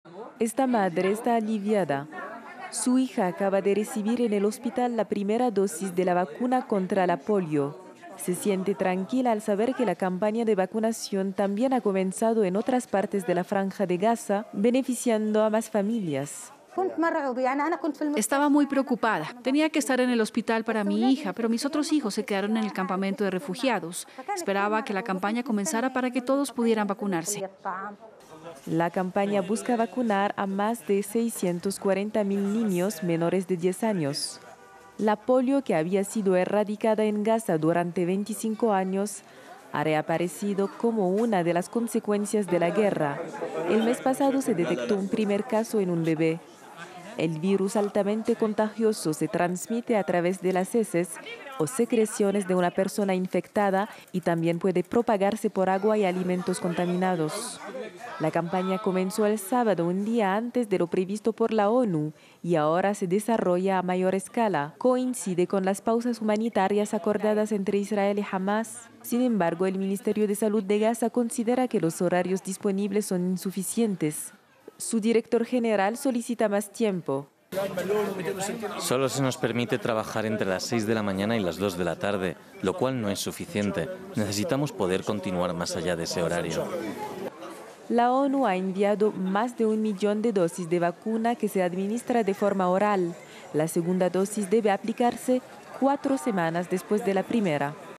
Comenzó la vacunación a gran escala contra la polio a los niños de la Franja de Gaza
La campaña de vacunación a gran escala contra la polio en la Franja de Gaza ya comenzó. Aunque se adelantó el sábado por la tarde en hospitales y algunos centros de vacunación, la campaña se expande a varias zonas del enclave palestino, coincidiendo con las pausas humanitarias, pactadas por Israel y Hamás la semana anterior.